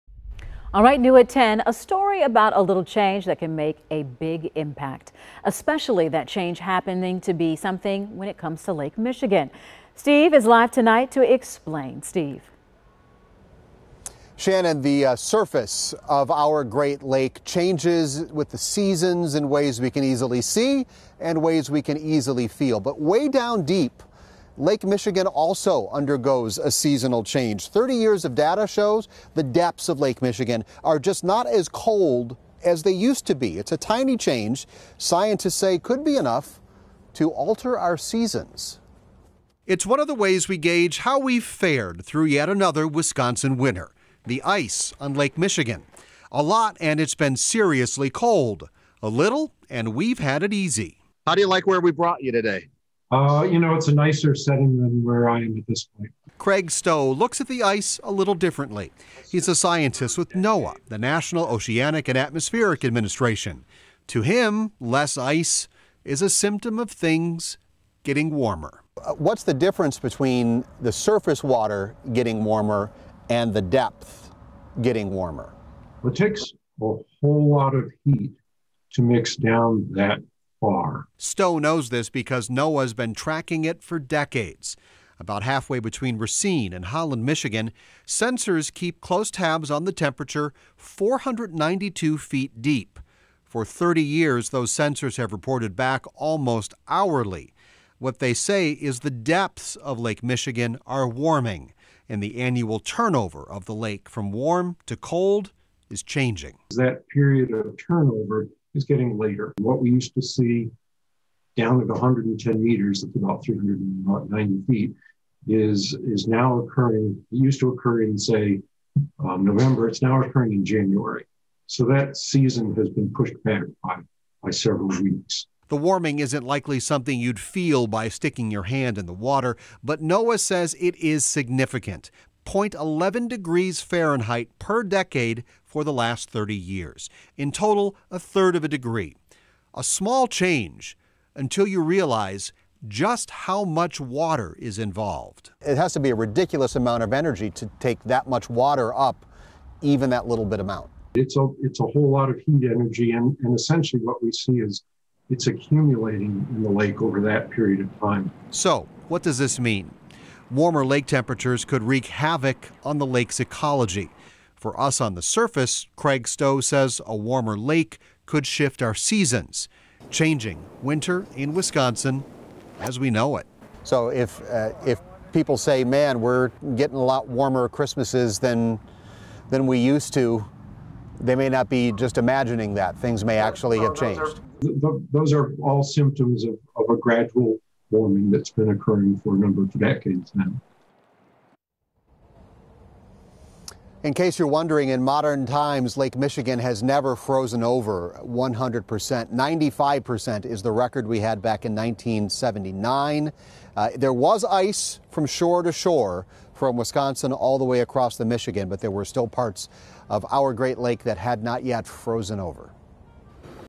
Warming Lake Michigan could be shifting Wisconsin’s weather
About halfway between Racine and Holland, Michigan, sensors keep close tabs on the temperature of Lake Michigan at a depth of 492 feet.  For 30 years, those sensors have reported back almost hourly.